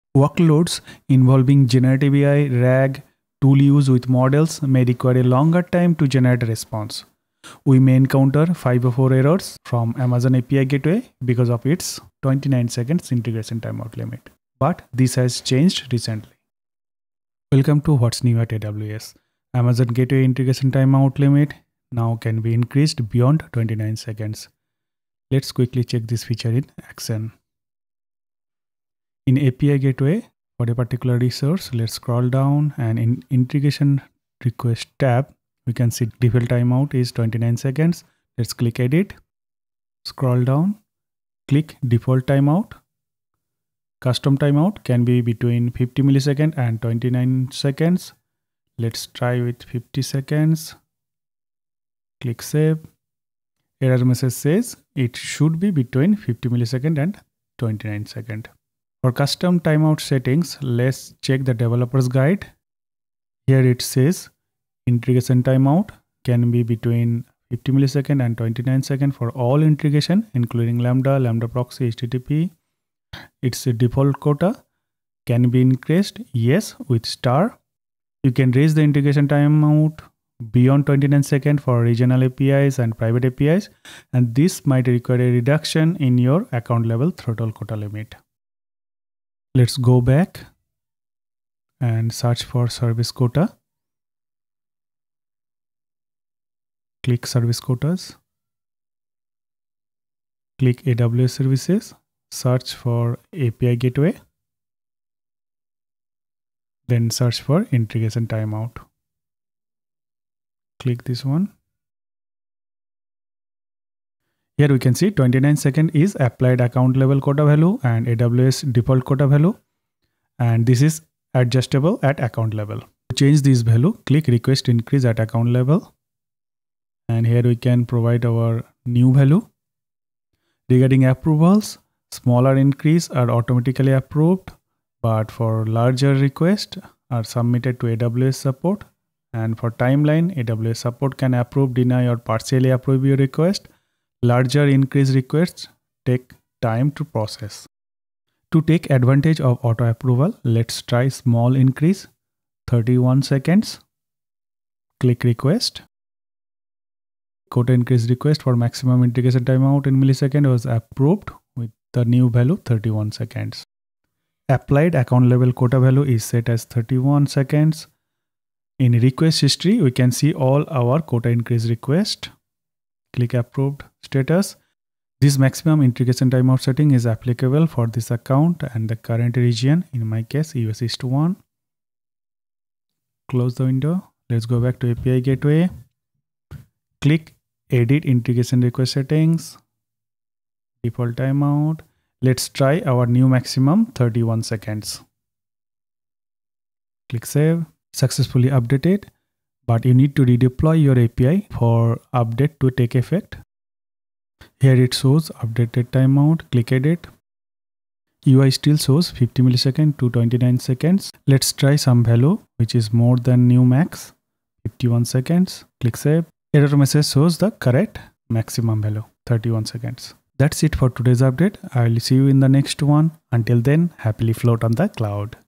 Gateway Integration Timeout limit increase beyond 29s
You can raise the integration timeout in Amazon API Gateway to greater than 29 seconds for Regional REST APIs and private REST APIs. This is useful to avoid HTTP 504 gateway timeout error in usecases (e.g. Generative AI)where it takes more time to generate response.

Check this video to see this feature in action!

00:00 504 Integration Timeout error
00:18 Increase Amazon API Gateway Integration Timeout
01:32 AWS Service Quotas
02:02 Quota increase request
03:05 Verify new Integration Request Timeout setting